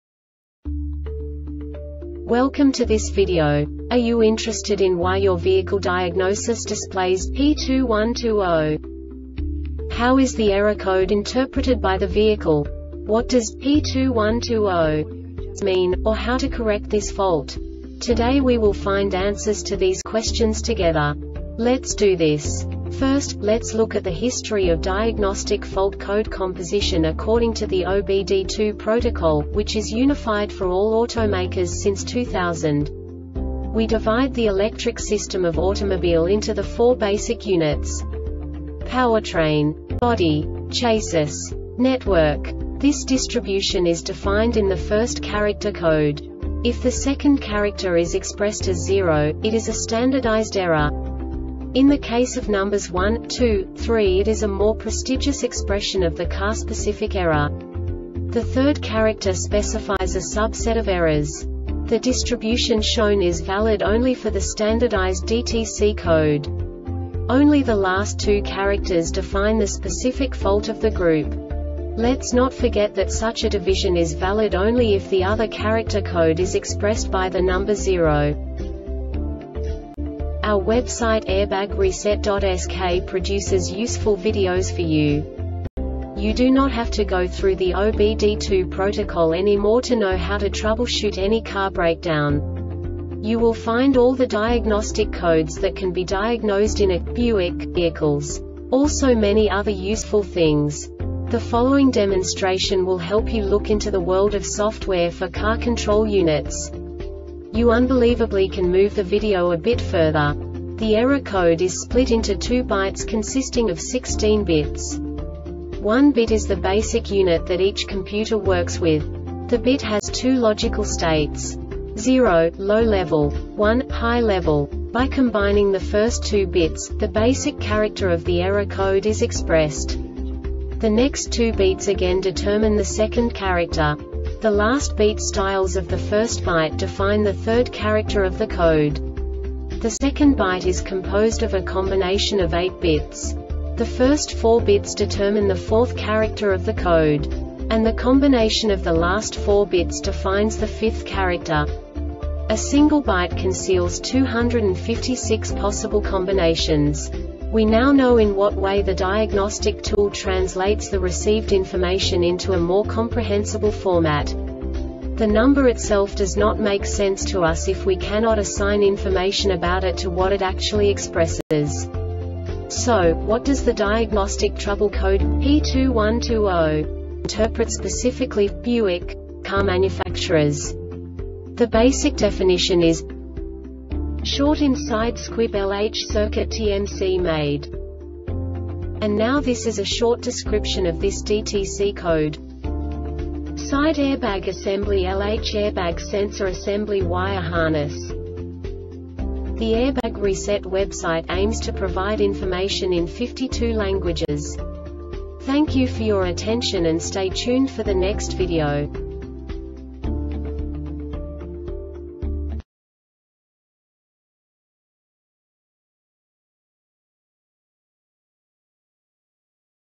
DTC Buick P2120 Short Explanation
Contents:
0:21 Basic DTC analysis according to OBD2 protocol standard.
1:48 Insight into programming
2:58 A diagnostically understandable description of the Diagnostic Trouble Code
3:05 Basic error definition

Make:
Buick
Code:
P2120
Definition:
Short In Side Squib (LH) Circuit (TMC Made)
Cause:
Side airbag assembly (LH)Airbag sensor assemblyWire harness